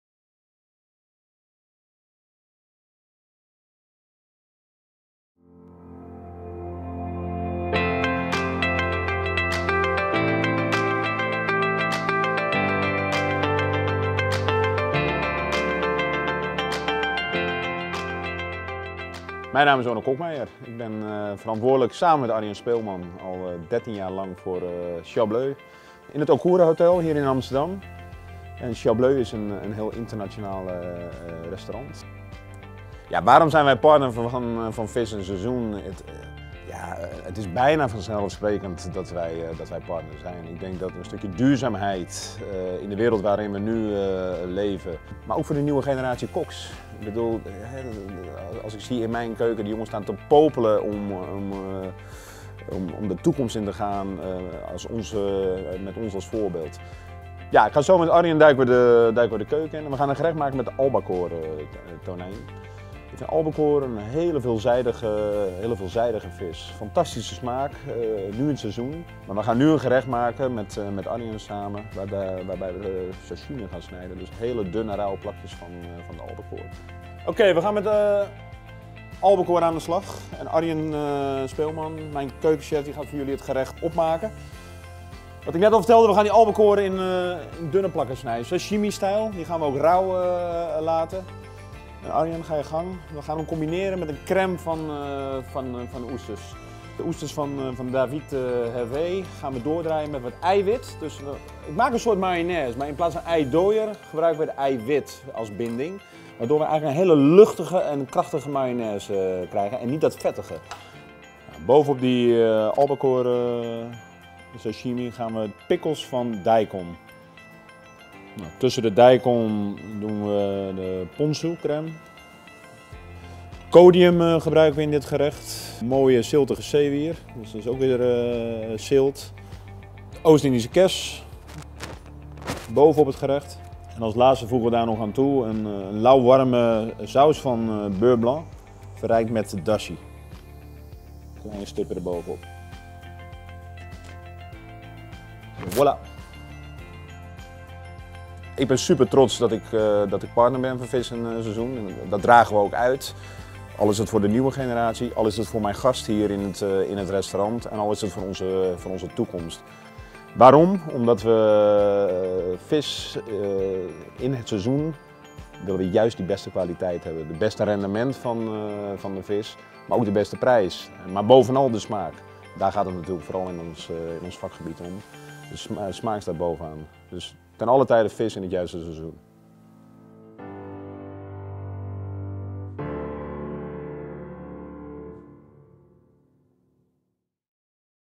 Partner van het 2e kwartaal: Restaurant Ciel Bleu
Vis&Seizoen partner van het kwartaal is dit kwartaal Restaurant Ciel Bleu, in Amsterdam.
Wij waren op bezoek bij Onno Kokmeijer en Arjan Speelman en vroegen hen waarom zij duurzaamheid hoog in het vaandel hebben staan.